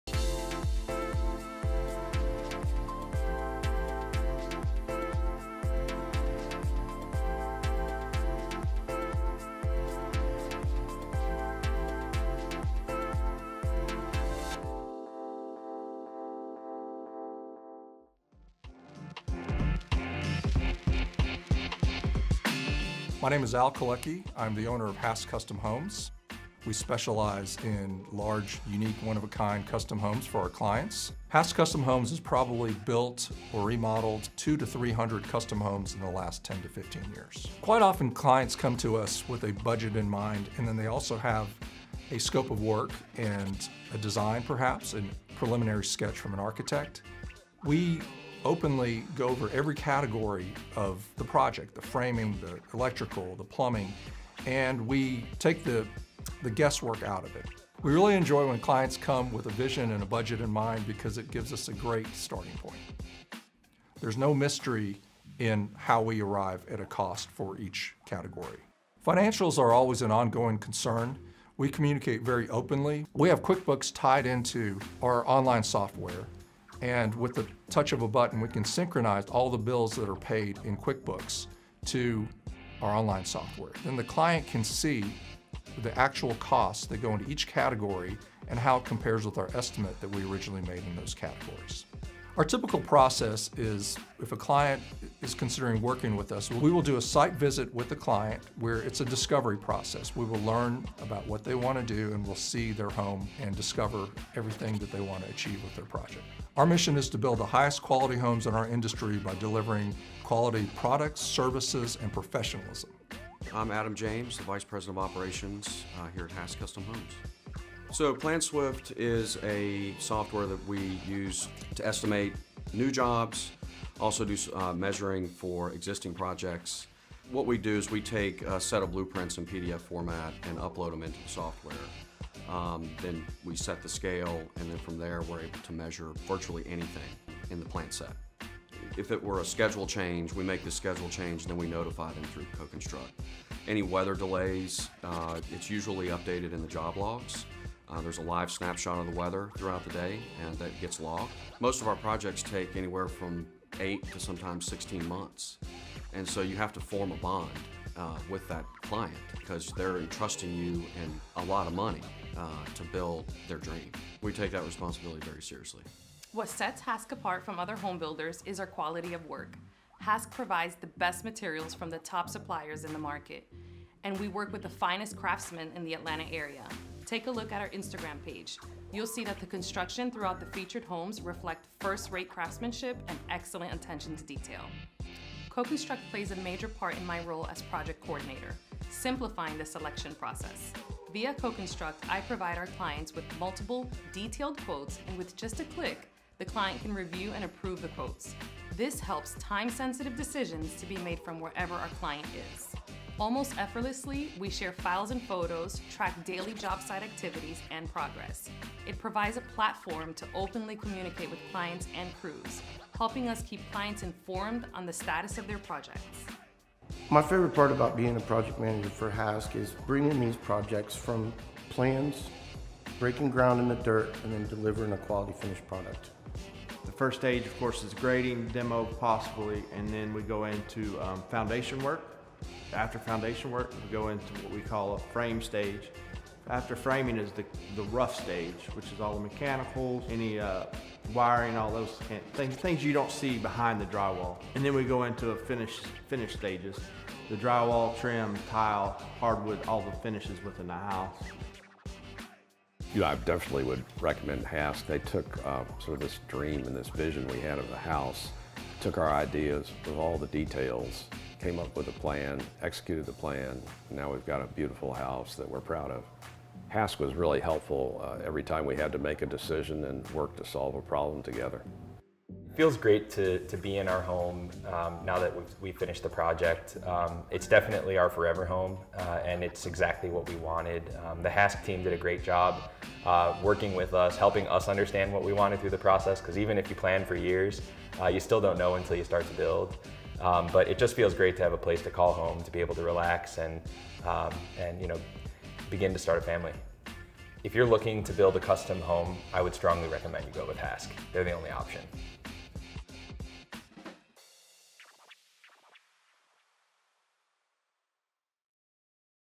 Hask Customer Experience
Video describing the customer
experience building or remodeling their home with Hask Custom Homes.